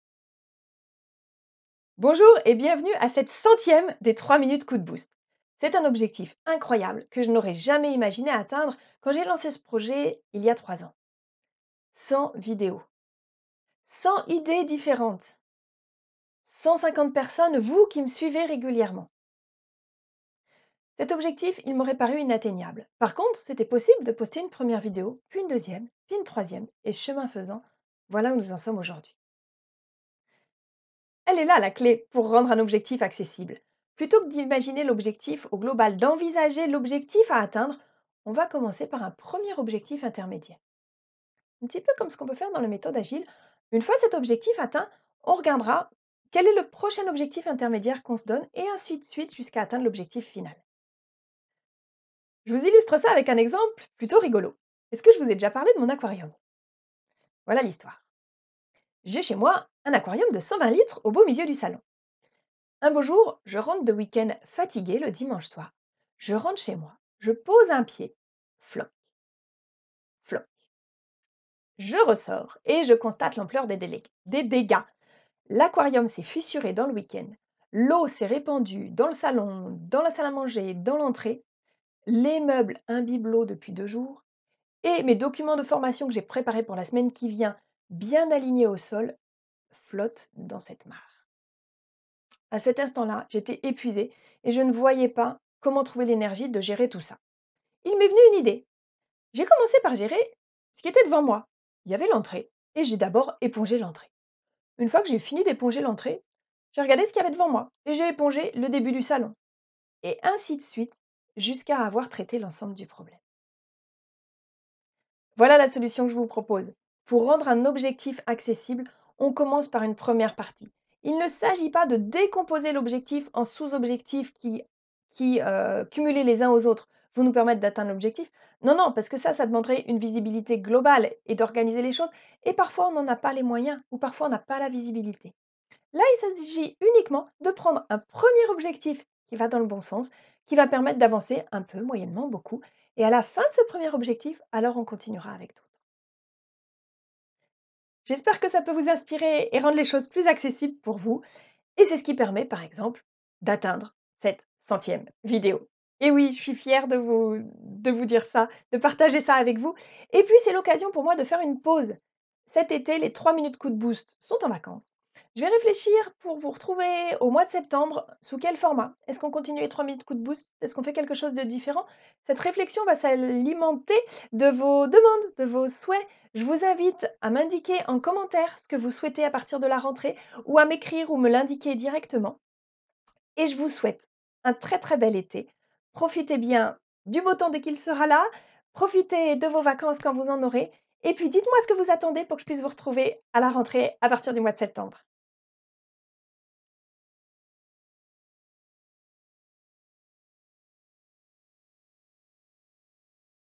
Les 3' coup de boost #100 💡 Rendre ses objectifs accessibles
📌 Les objectifs à atteindre paraissent parfois une montagne insurmontable…

👉 Comment les rendre accessibles en tenant compte de nos limites ?
👉 Quel piège éviter ?
👉 Avec une illustration aquatique ;-)

Isabelle Morin Formatrice et Coach en management et développement personnel... mais pas que !